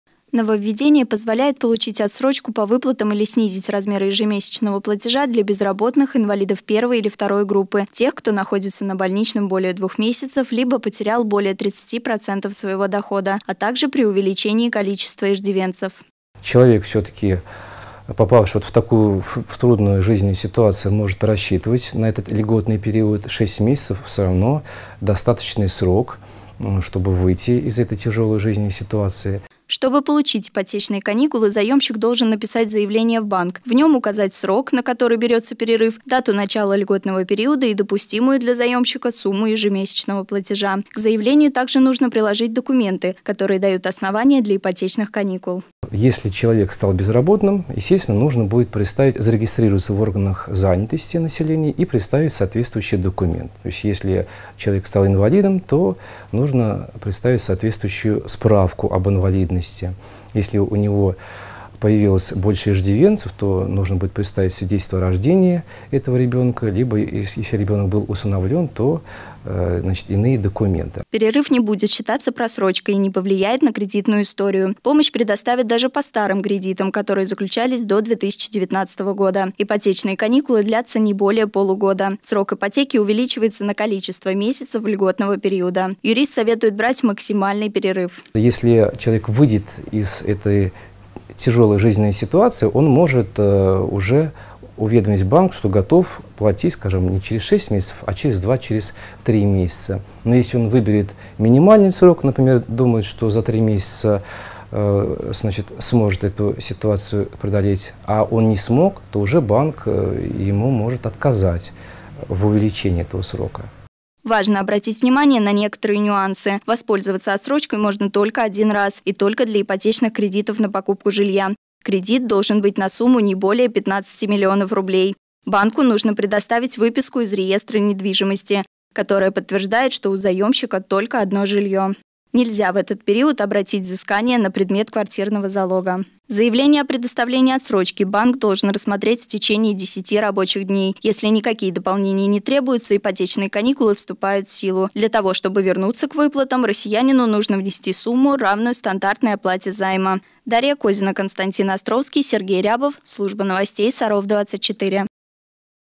Ипотечные каникулы
Вступил в силу закон об ипотечных каникулах. Кто может получить отсрочку, на какой срок и какие документы для этого нужны?